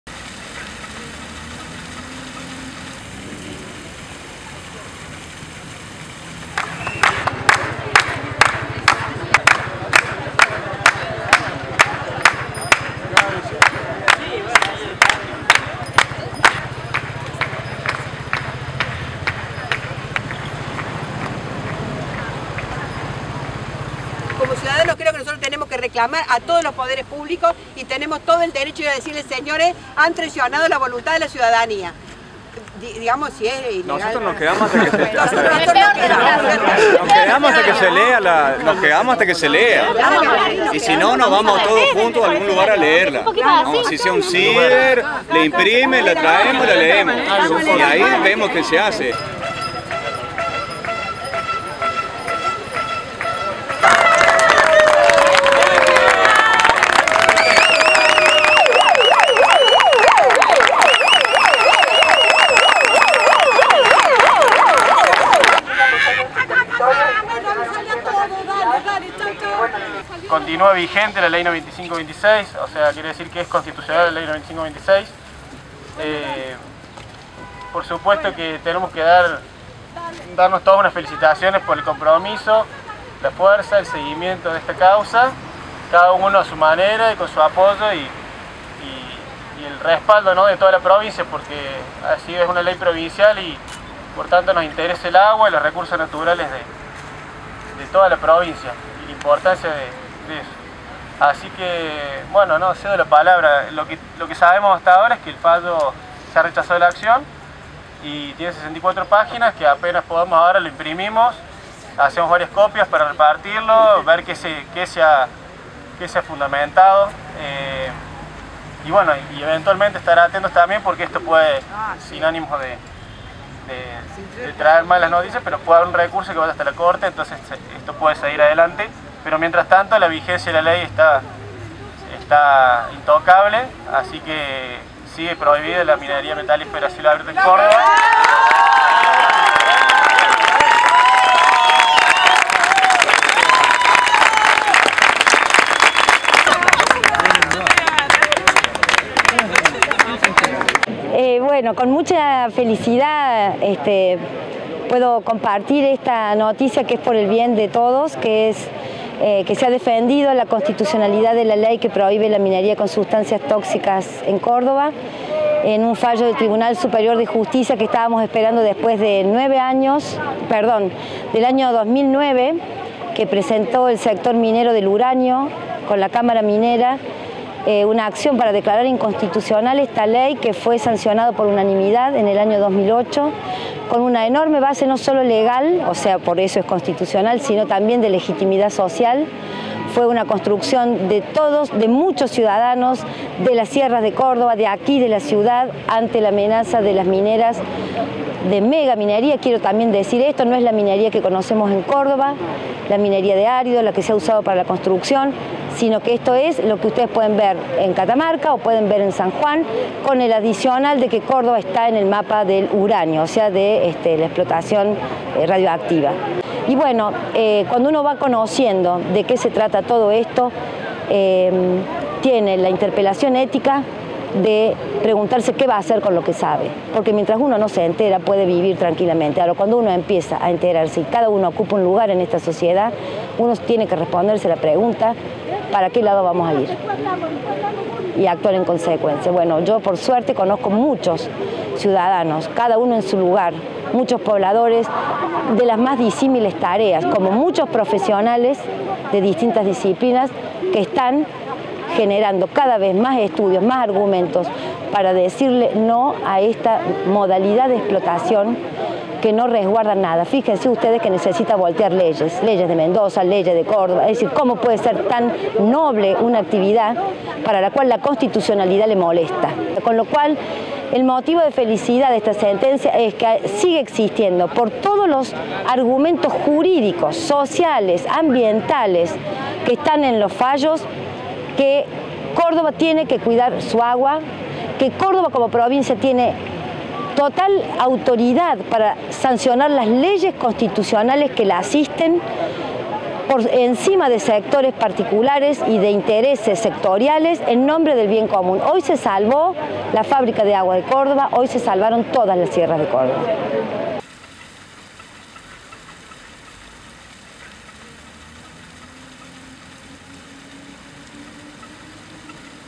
Córdoba le dice NO a la minería contaminante
El Tribunal Superior de Justicia rechazó el pedido de inconstitucionalidad de la ley 9526. En Córdoba, las actividades mineras contaminantes continúan siendo ilegales. Entrevista a Mirta Antonelli.